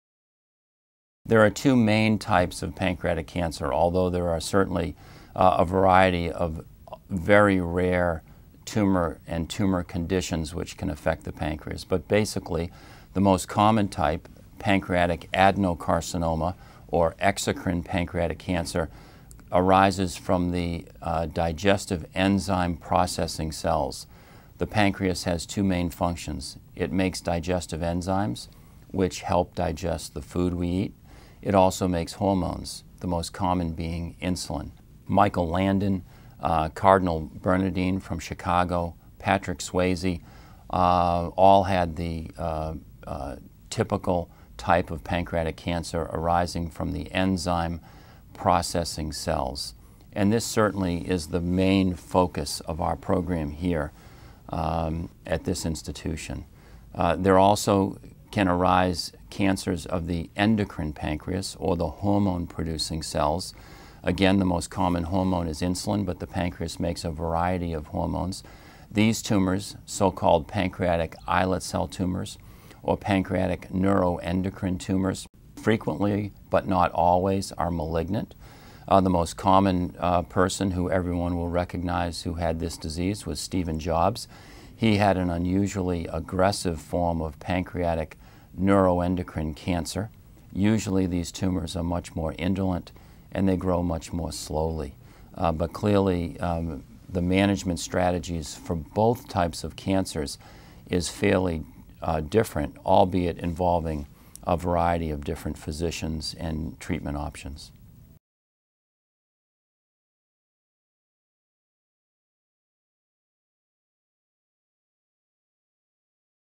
What are the types of pancreatic cancer? (Douglas Evans, MD)
Douglas Evans, MD, Medical College of Wisconsin surgical oncologist, describes the different types of pancreatic cancer - adenocarcinoma and neuroendocrine (islet cell) tumors. Dr. Evans is part of the pancreatic cancer treatment team at Froedtert & the Medical College of Wisconsin.